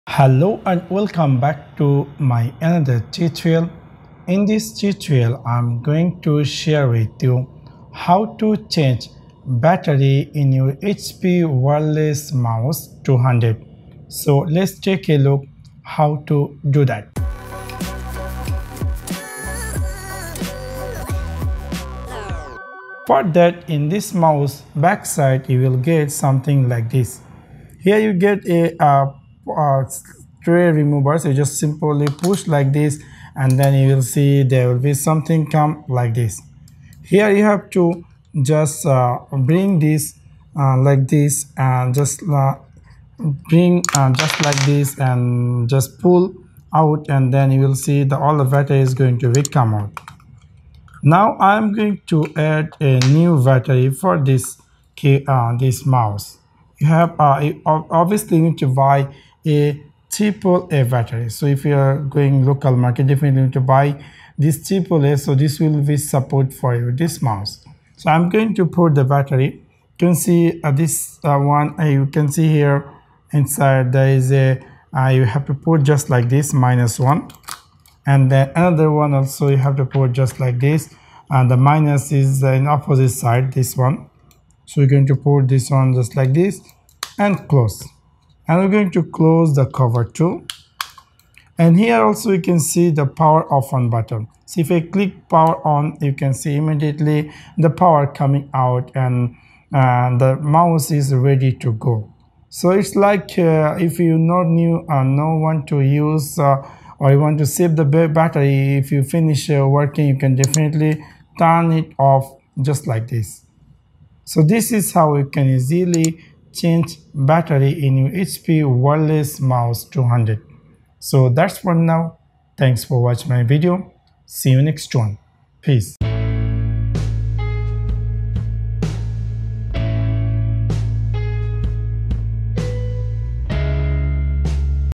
How To Change Battery In HP Wireless Mouse 200
The HP Wireless Mouse 200 Black is comfortable to use thanks to its shape and size and relieves the wrist. You won't be bothered by wires becoming entangled on your desk because the mouse is wireless. The wireless connectivity of 2.4GHz gives you a range of up to 10 meters and a 360-degree angle. Thanks to the 1,000DPI, you can smoothly move through all your files and pages with the mouse cursor.
In this video I am going to share with you How To Change Battery In HP Wireless Mouse 200.